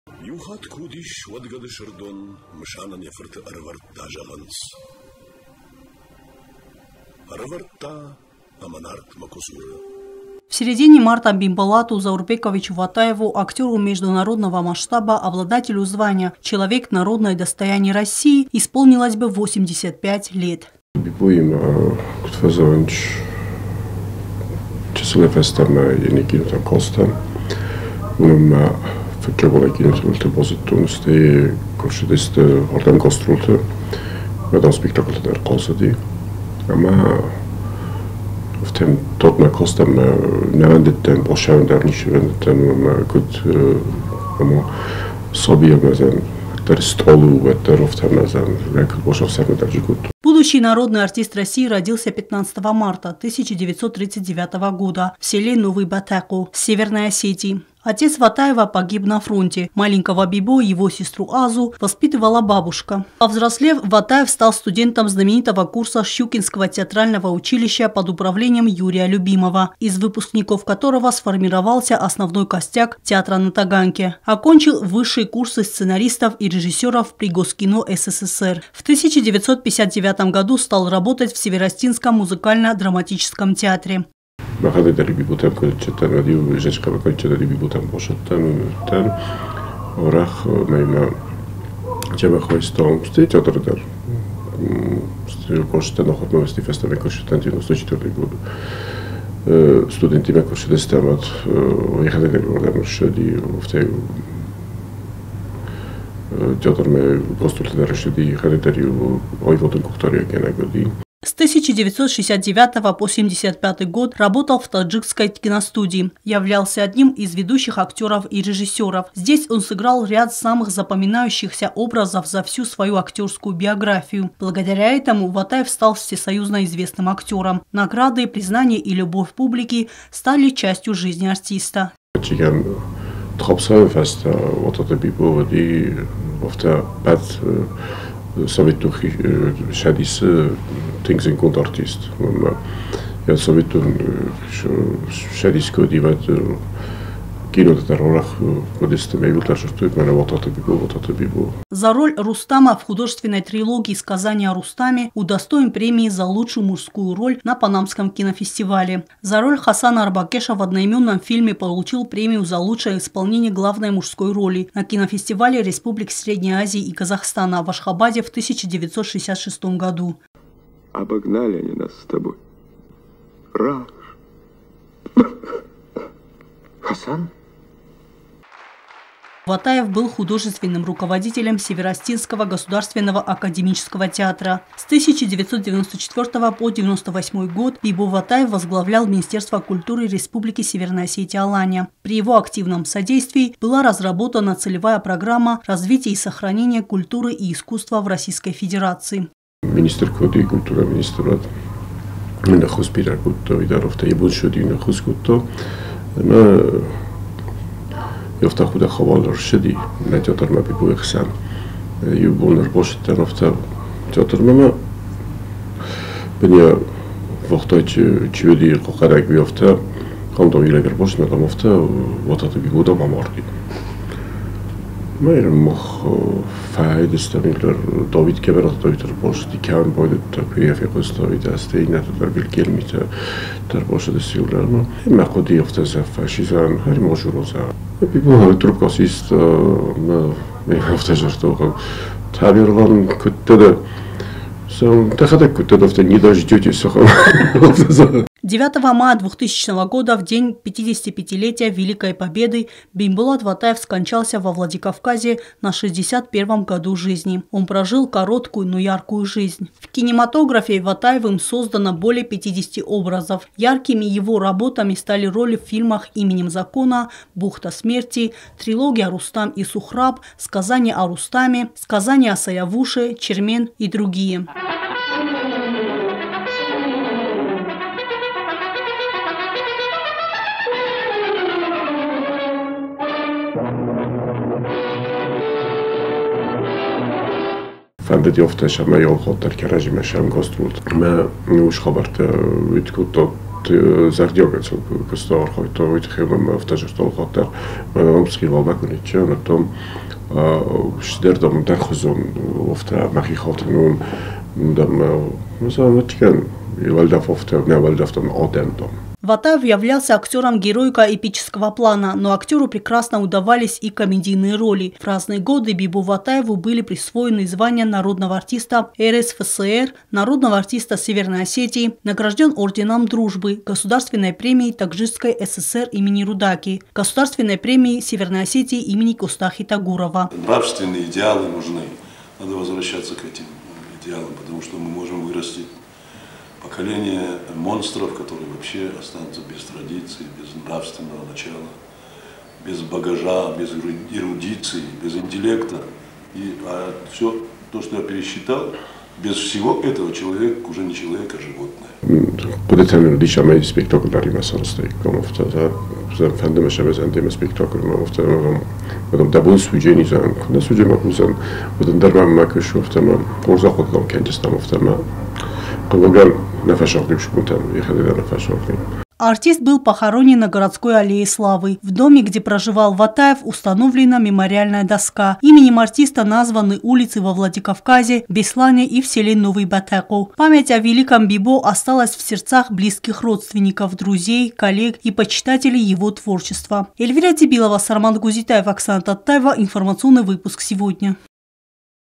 В марте месяце исполнилось 85 лет со дня рождения советского артиста театра и кино Бимболата Ватаева
Талантливый актер прожил короткую, но яркую жизнь. В кинематографе Ватаевым создано более 50 образов. Есть и режиссерские работы. Он являлся художественным руководителем Северо-Осетинского государственного академического театра. Ватаев - актёр героико-эпического плана, но ему удавались и комедийные роли. Созданные им образы на сцене до сих пор незабываемы среди почитателей его таланта.